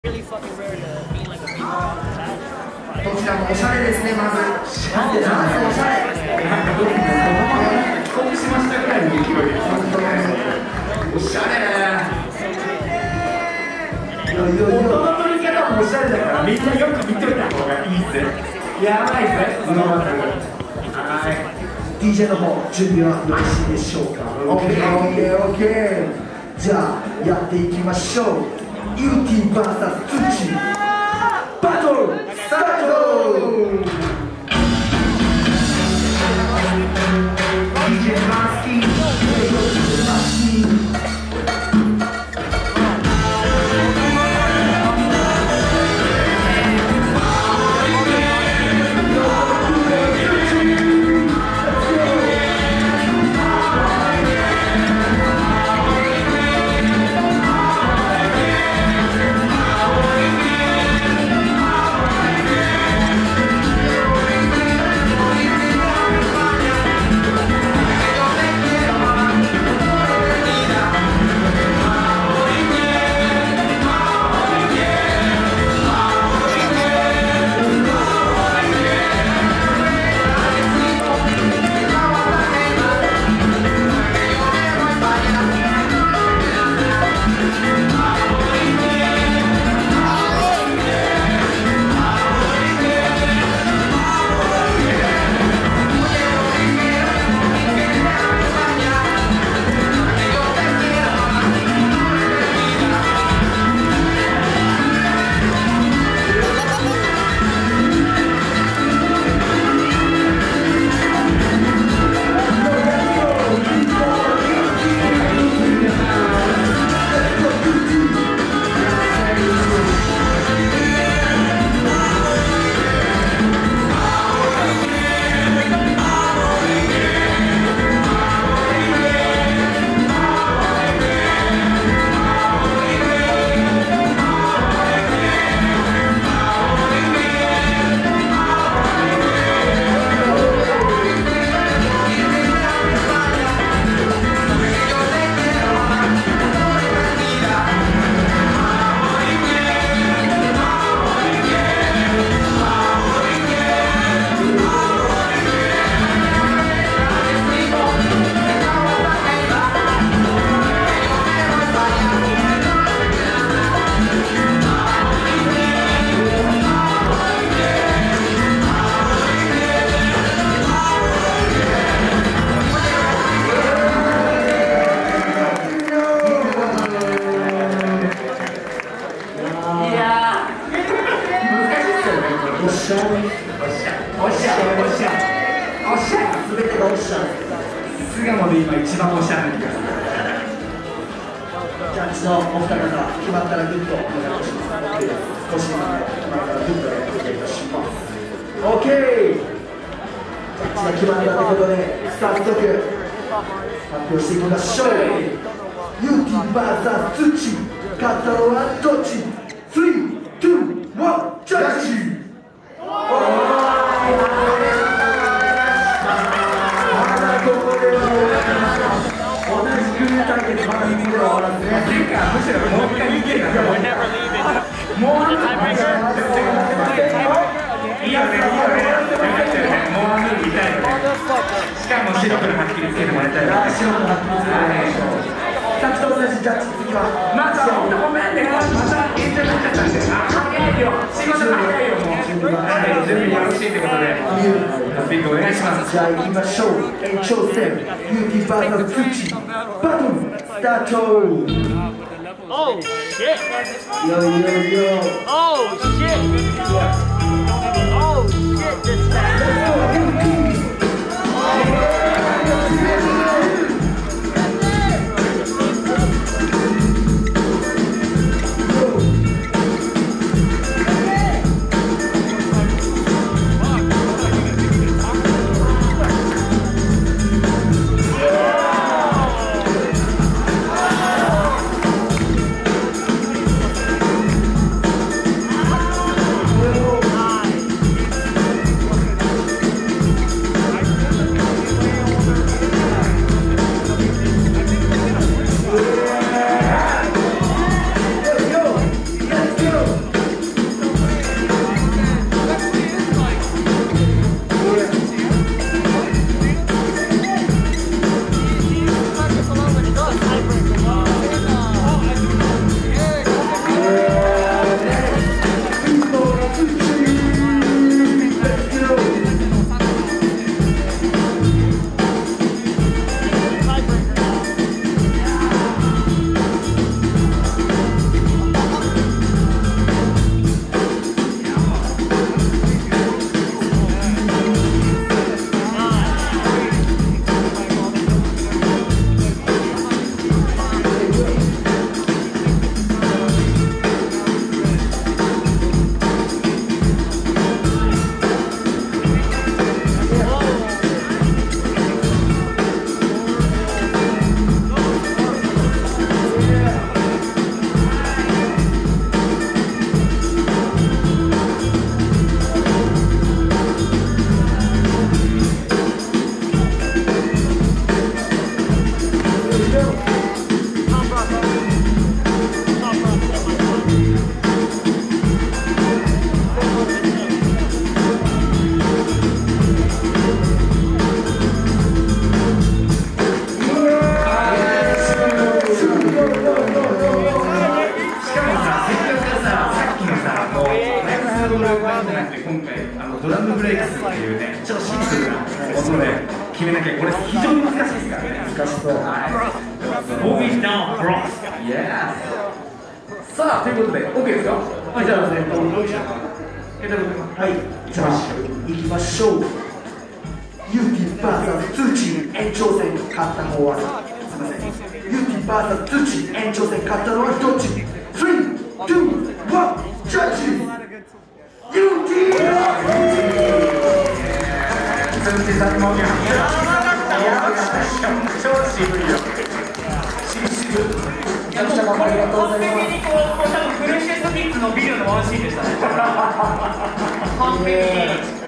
2UCCI vs YOUTEE [TOPROCK TOP8] // FN JAM 2022 - FOUND NATION 20TH ANNIVERSARY -
FN JAM 2022 - FOUND NATION 20TH ANNIVERSARY
TOPROCK 1VS1 BATTLE

Winner: YOUTEE